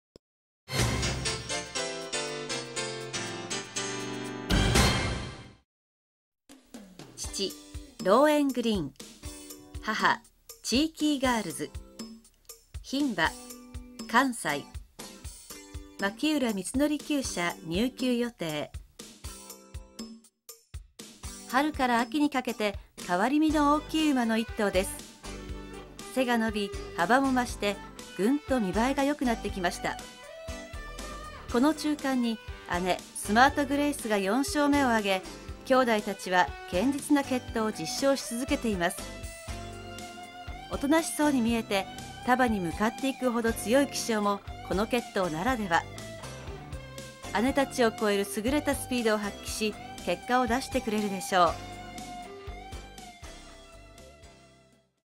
PEGASUS11 2017年秋
ユニオンオーナーズクラブ　2017年度1歳募集馬
PEGASUS11　牝1歳
父ローエングリン　母チーキーガールズ
栗東・牧浦充徳厩舎予定　飛野牧場生産
募集価格800万円　1/200口40,000円